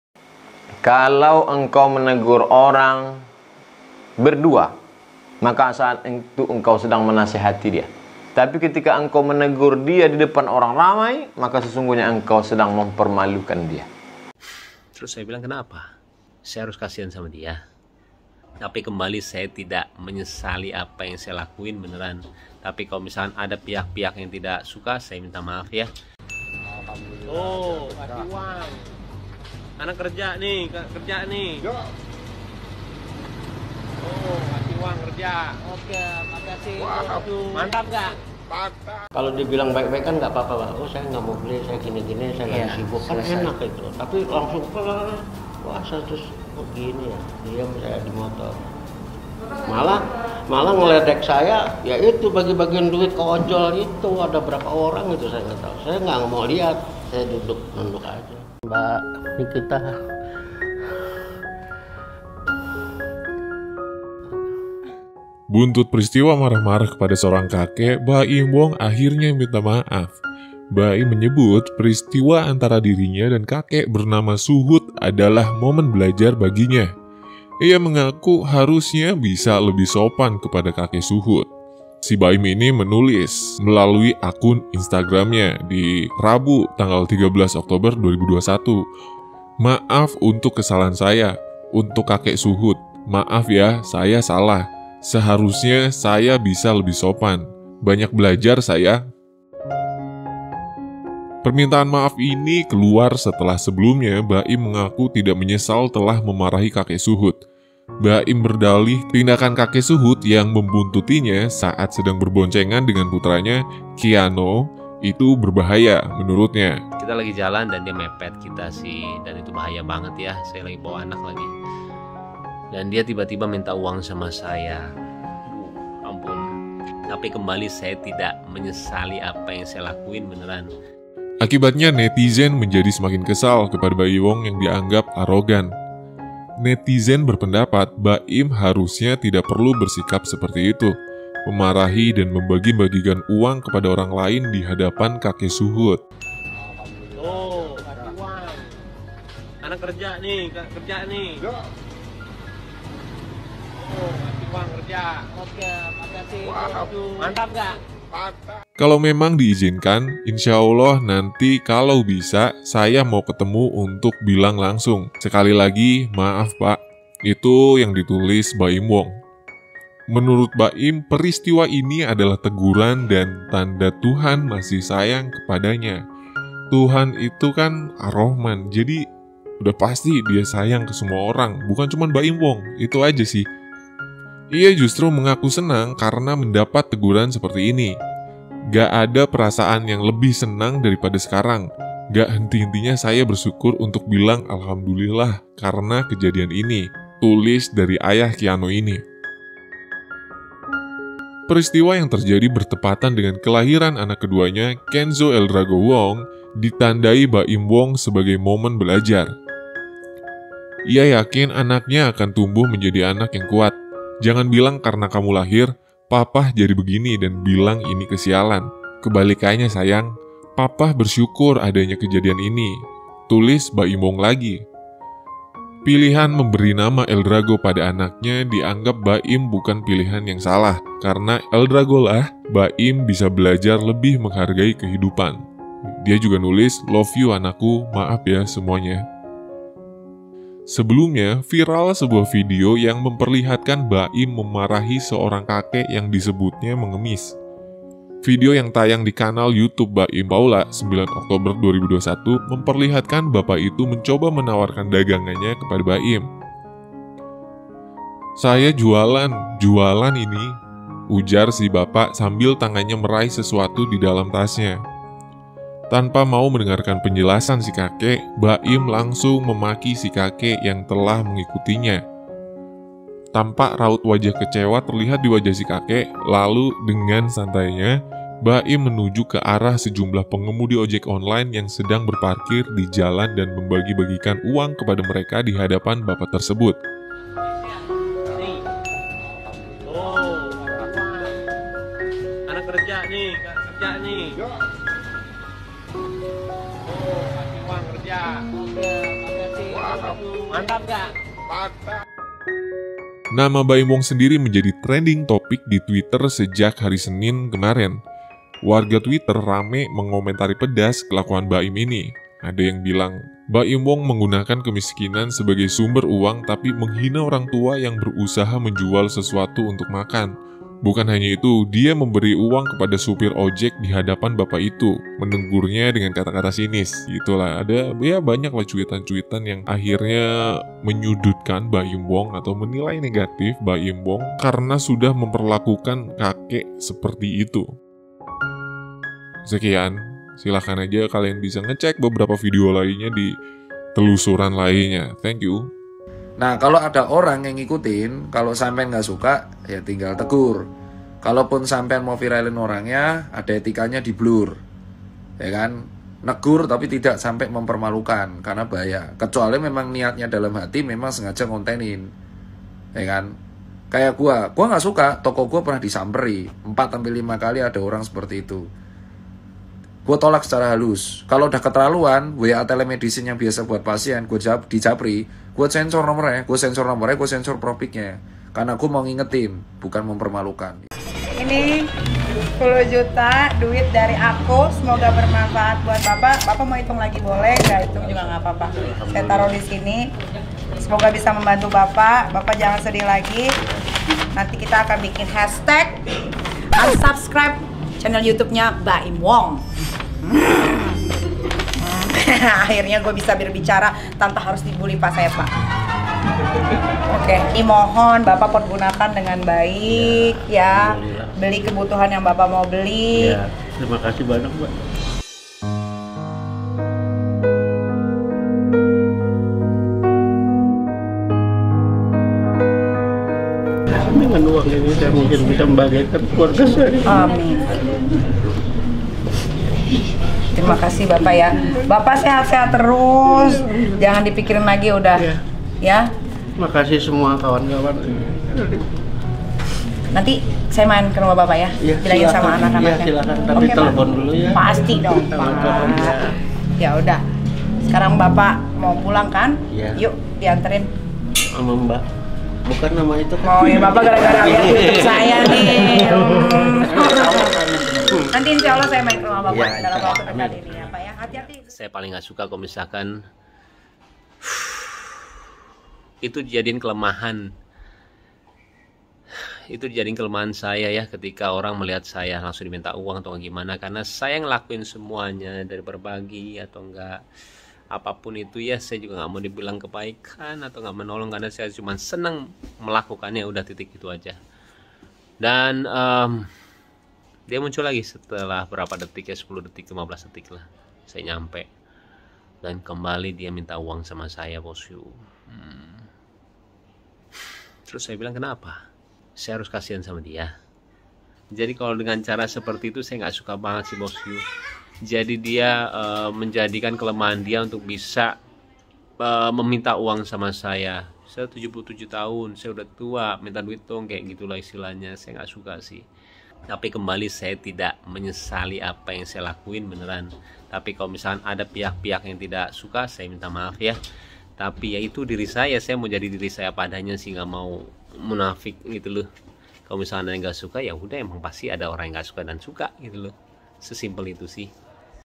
BAIM WONG MARAHI KAKEK SUHUD‼️ UAS AJARKAN CARA YANG BENAR‼️ NIKITA IKUT COLONG START
Baim Wong memarahi, menghina Kakek Suhud dengan sebutan "Pengemis" dan mempermalukannya di depan umum, di depan ojol, di depan penonton youtube-nya sebelum akhirnya di-takedown videonya.

Rekam jejak video klarifikasinya ternilai tidak menyesal saat minta maaf.

dr. Tirta juga angkat bicara di menit ke 5:47 (nanti aja nontonnya, biar gak kelompat beritanya)